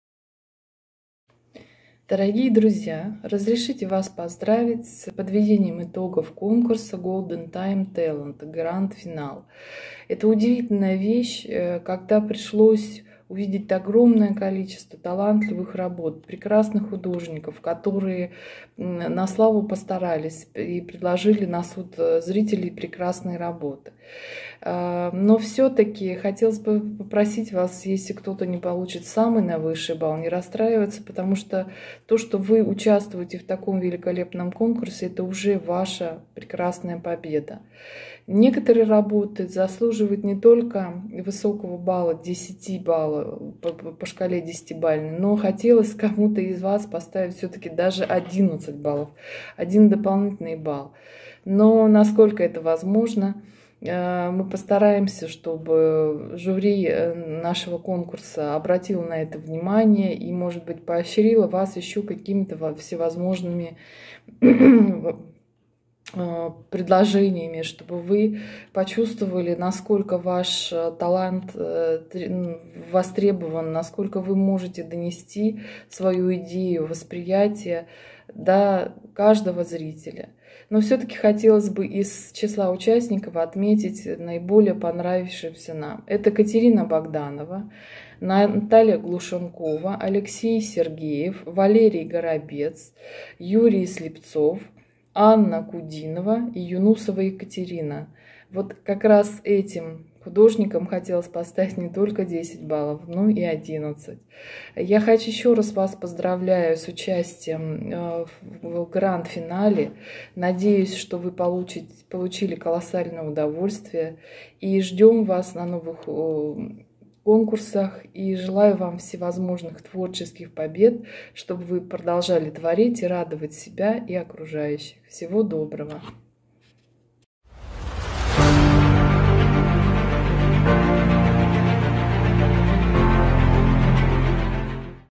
Видео оценка конкурсных работ GRAND FINAL - жюри конкурса Golden Time Talent - ART GALERY EVERYDAY
Дорогие друзья!
Публикуем видео оценку GRAND FINAL (PAINTER) жюри конкурса Golden Time Talent - ART GALERY EVERYDAY 🇷🇺  Russia